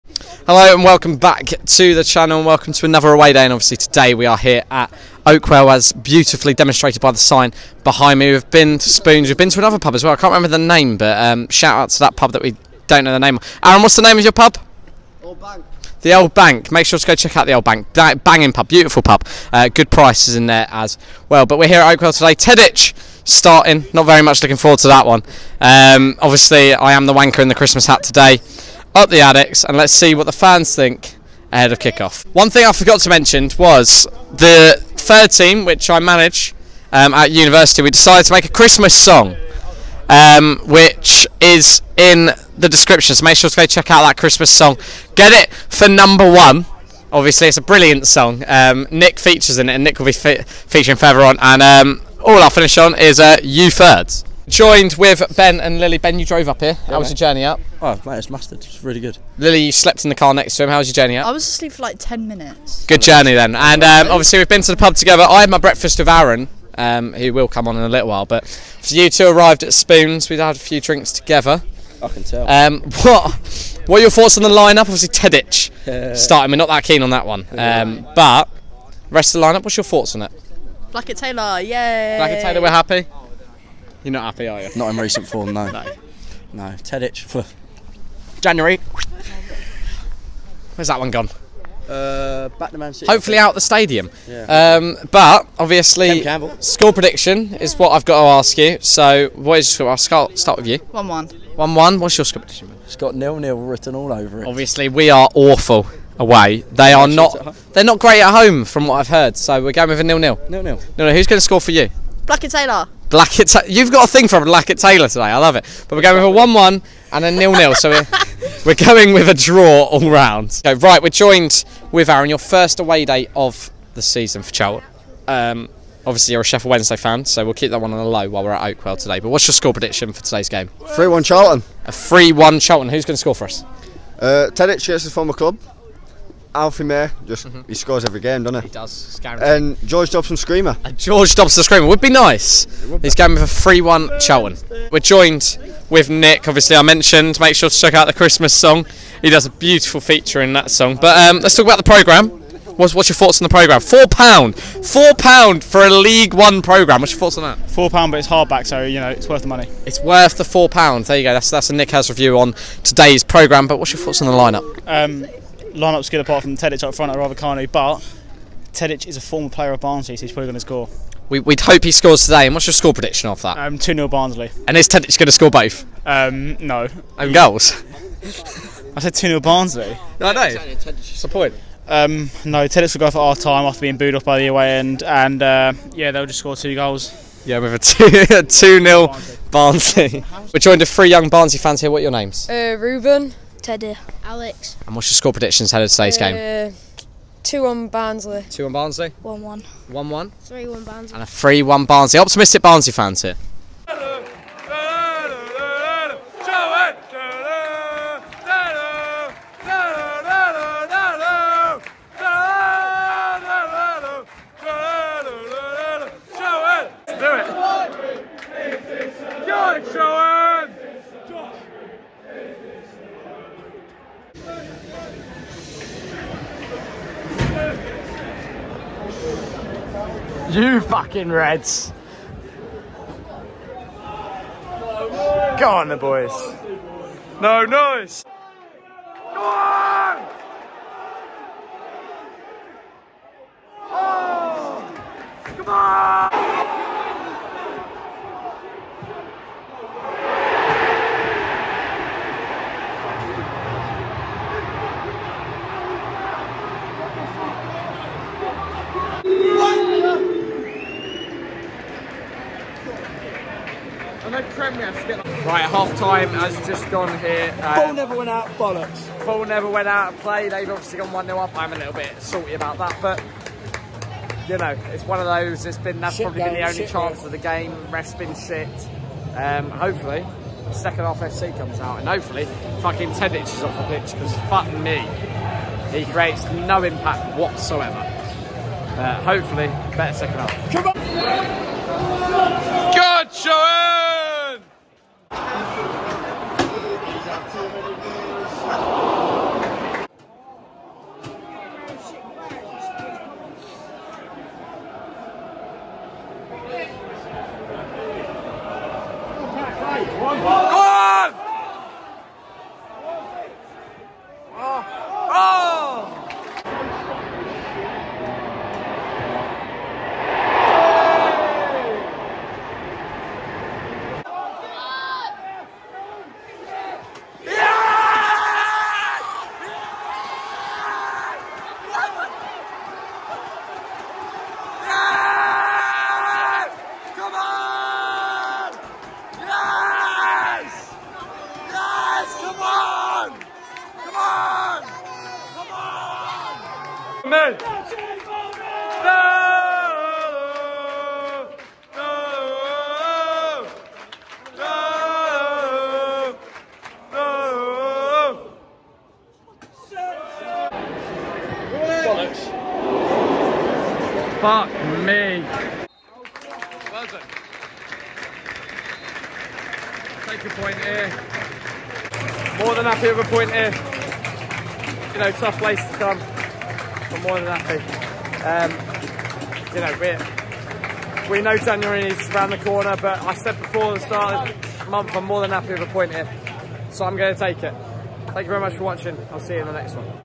ADDICKS COMEBACK TO GAIN A POINT ON THE ROAD // BARNSLEY 1-1 CHARLTON ATHLETIC
Not an awful performance on the road but happy with the point. Some interesting officiating throughout this game as always!

Sorry for the length of the vlog- wasn’t too much content to get today and was a bit of a disappointing game on that front!

Let’s hit 25 likes on this one!